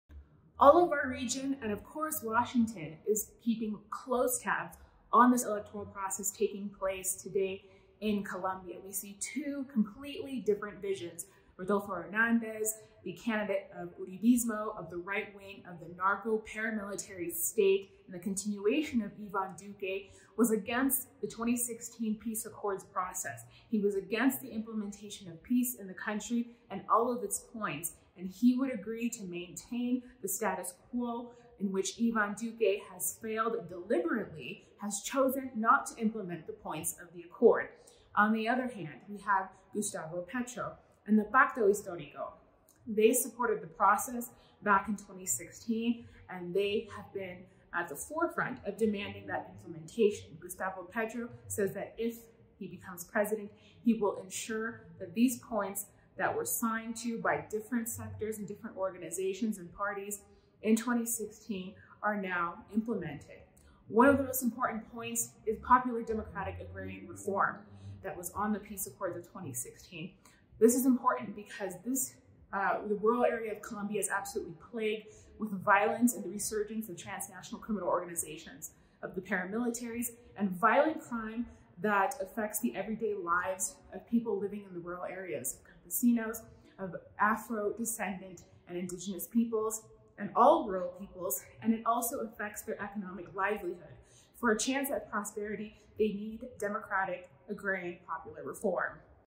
Orinoco Tribune - Camila Escalante from Kawsachun News - On Colombia's Presidential Elections
An update at Sunday noon (June 19) on second round of presidential elections by Camila Escalante from Bolivia.